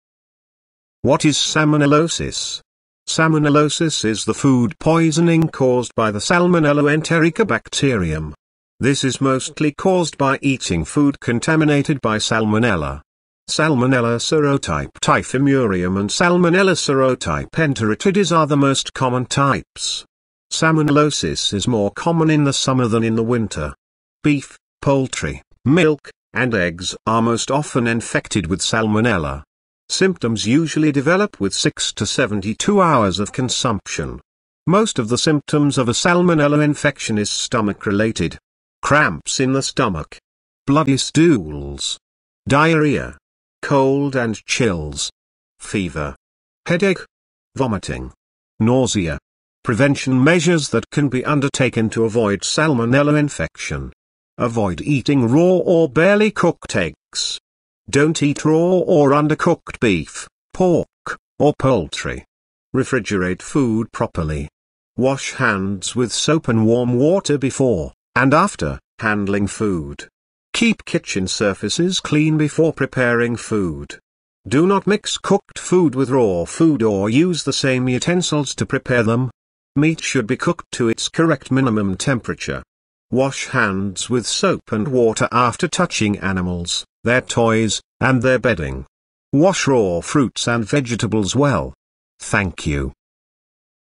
What is Salmonellosis?
Salmonellosis is the food poisoning caused by the Salmonella enterica bacterium.
This is mostly caused by eating food contaminated by salmonella.